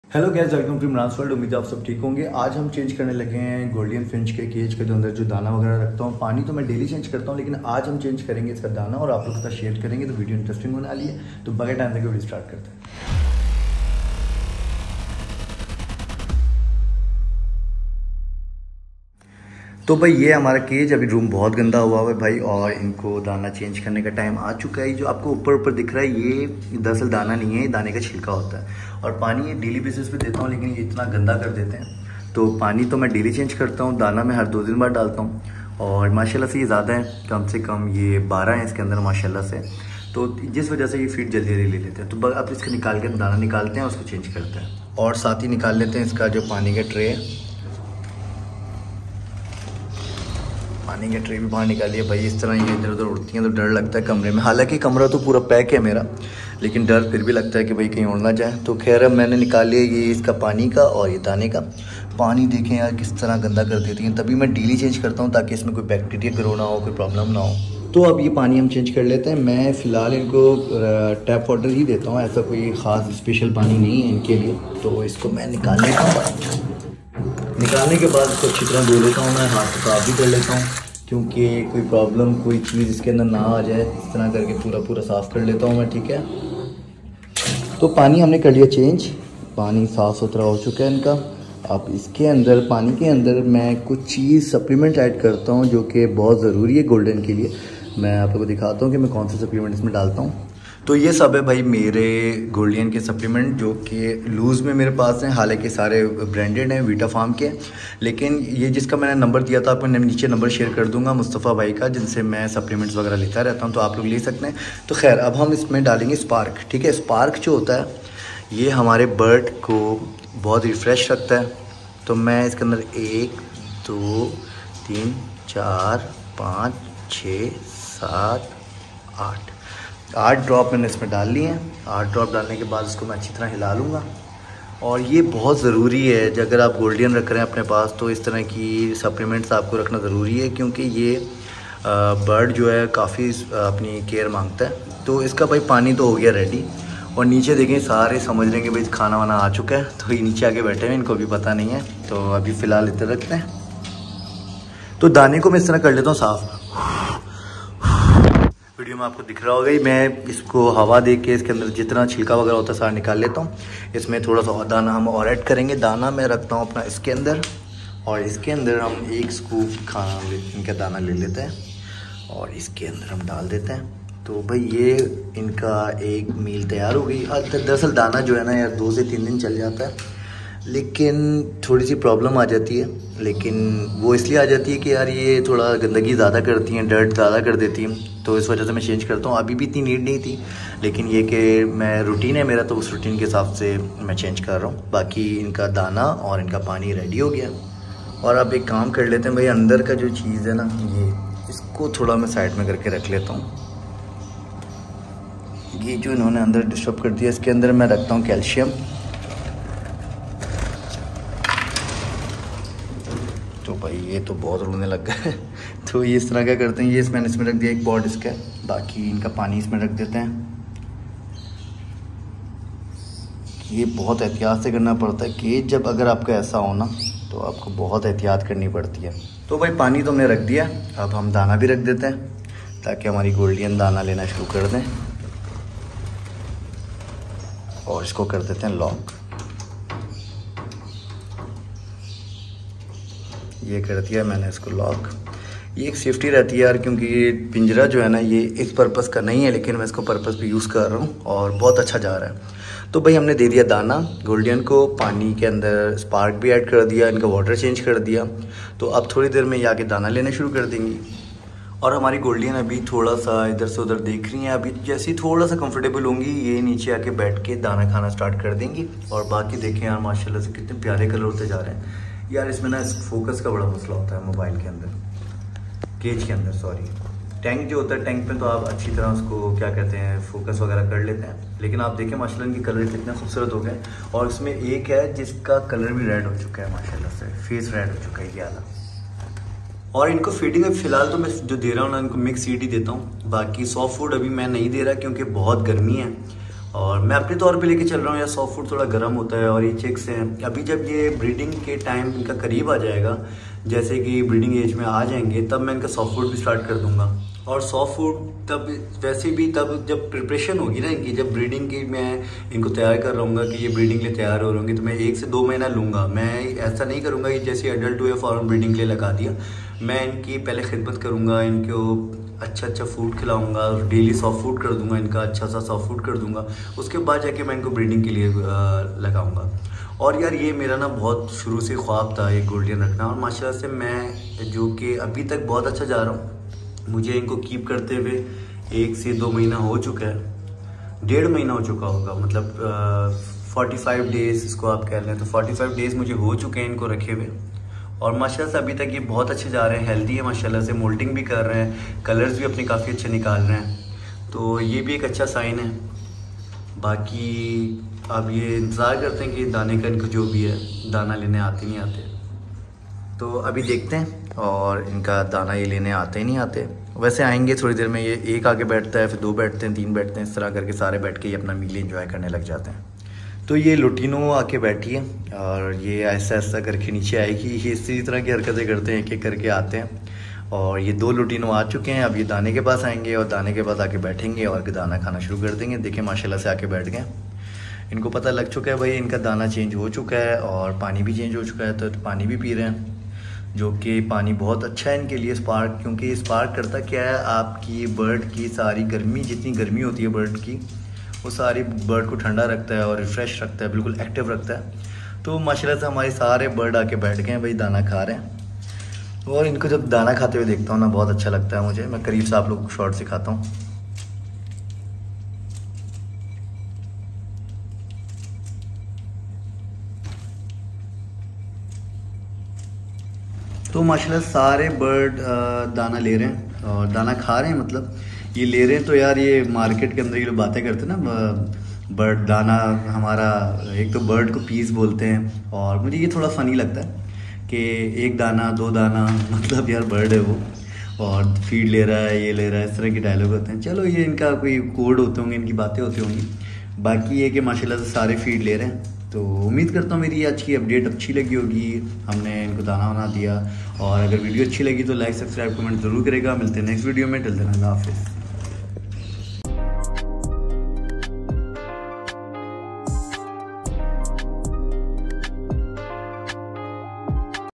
Changing the Feed in My Gouldian Finch Cage in Hindi/Urdu - Imran's World
In today's video, I'll be showing you how I change the feed in my Gouldian Finch cage. Keeping our feathered friends healthy and happy starts with providing them the right diet, and I'll walk you through my process step-by-step.

Whether you're a seasoned bird owner or a newbie, this video will help you understand the essentials of Gouldian Finch care.

By the end of this video, you'll have a deeper appreciation for the beauty and elegance of Gouldian finches.

If you have any questions or tips, leave them in the comments below. Enjoy the beauty of Gouldian finches!